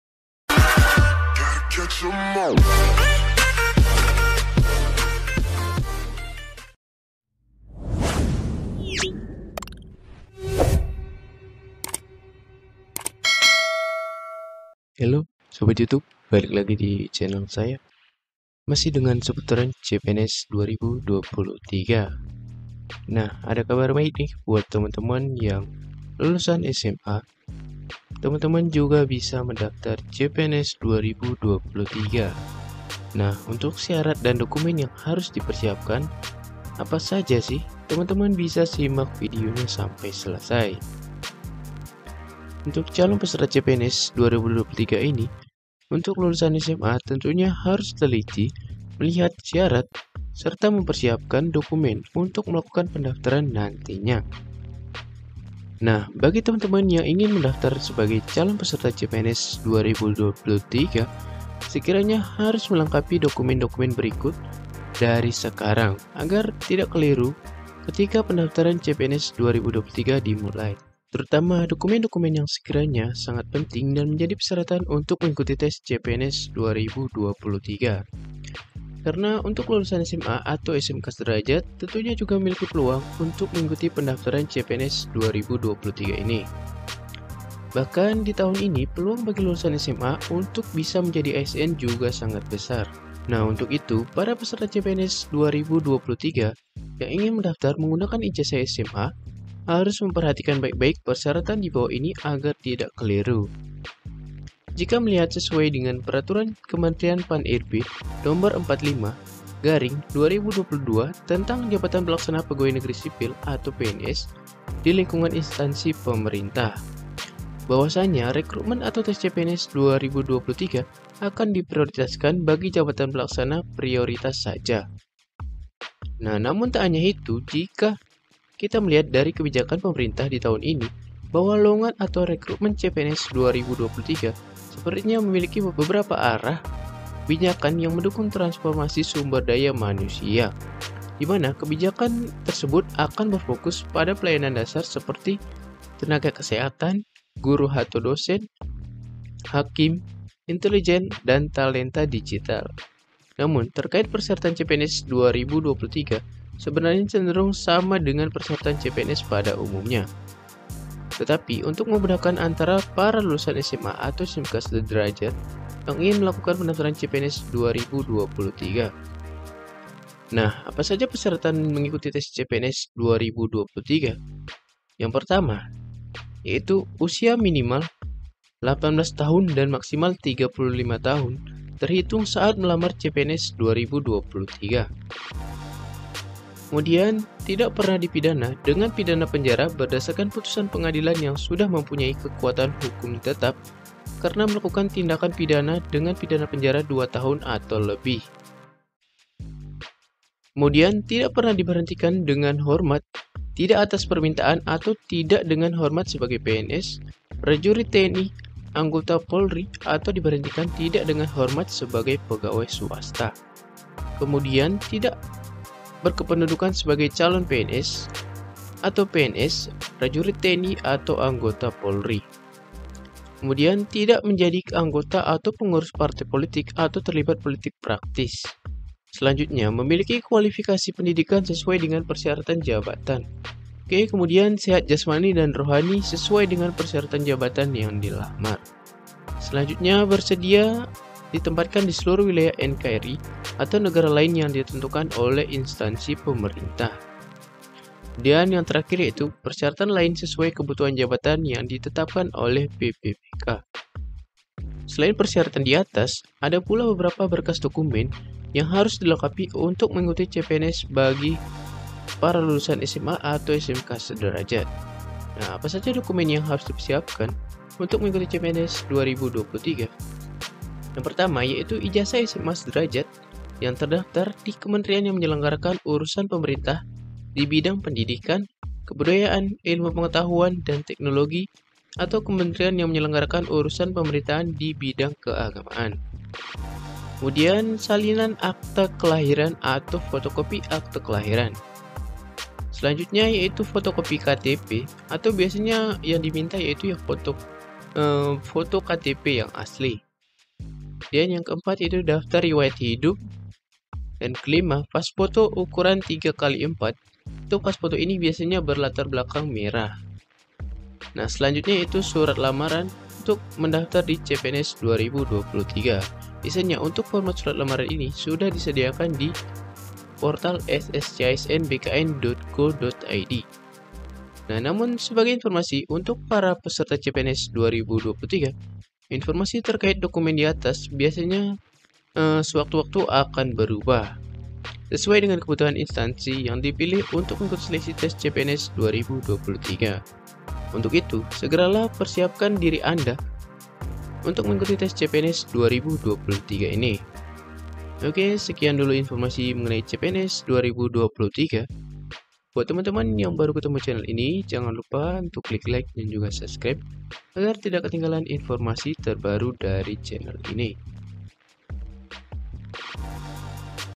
SYARAT DAN DOKUMEN YANG HARUS DISIAPKAN UNTUK CPNS 2023 !! PENTINGG UNTUK DI SIMAK!!
Di dalam video ini berisikan tentang syarat dan dokumen-dokumen yang harus disiapkan untuk mengikuti cpns 2023.
Temen2 bisa simak video-nya
Semoga informasi-nya bermanfaat buat temen-temen yang isudah lama menunggu cpns di buka kembali.